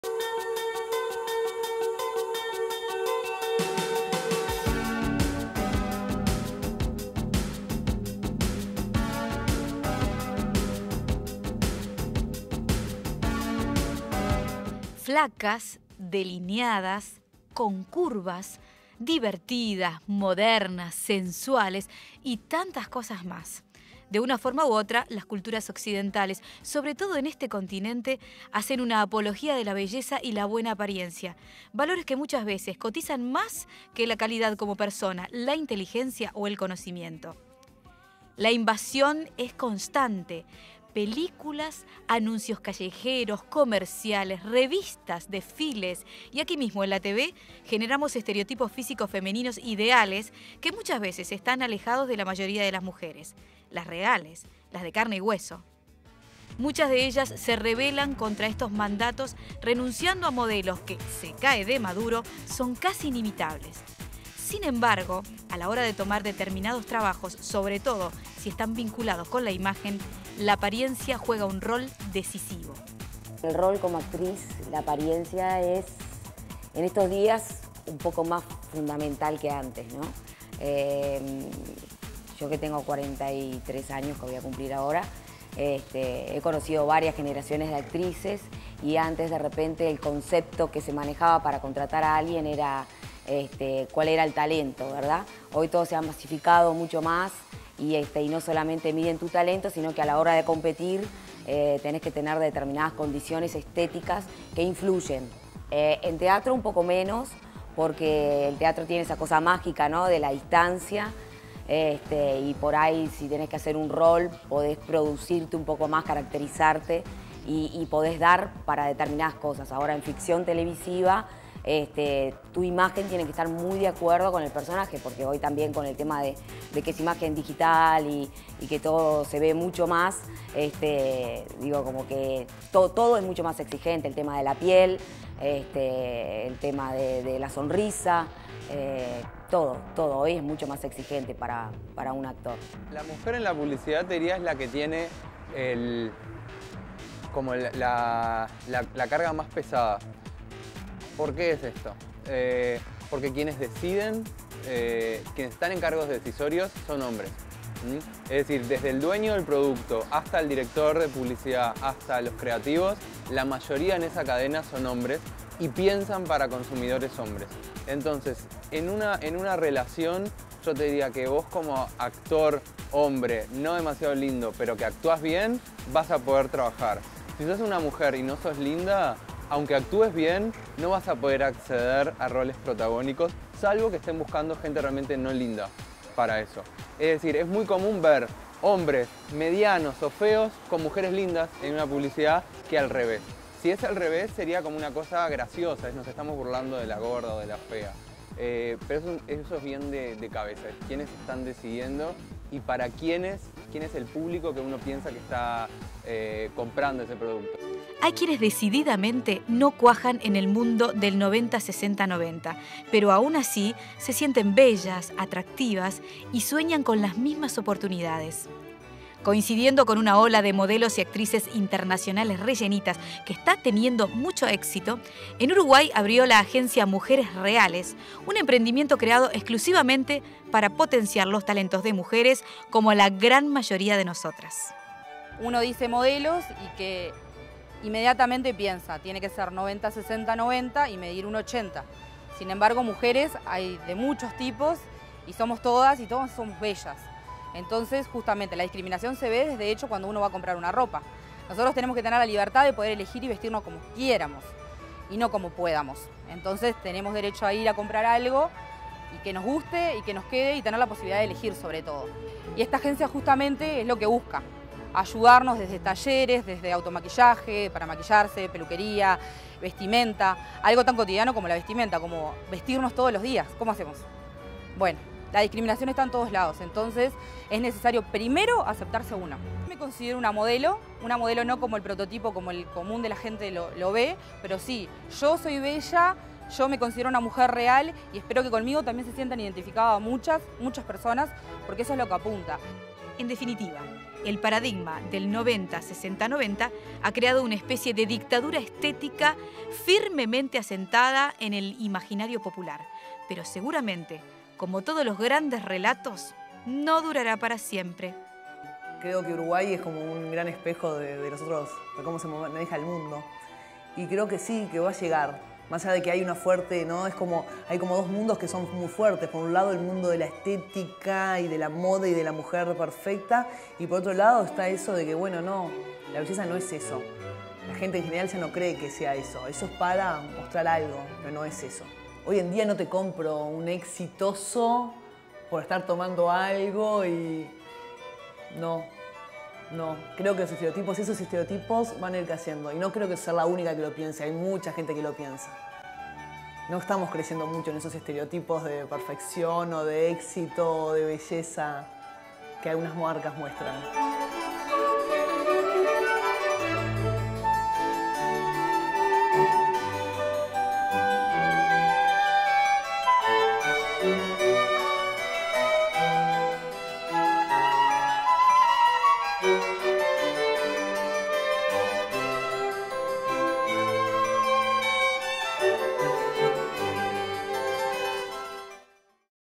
Informe especial: la dictadura de la estética
¿Cuánto pesan los cánones estéticos? ¿Quién y cómo determina qué mujeres son bellas? Armamos un informe especial, entrevistando a profesionales vinculados al tema de la imagen, para indagar más sobre estos temas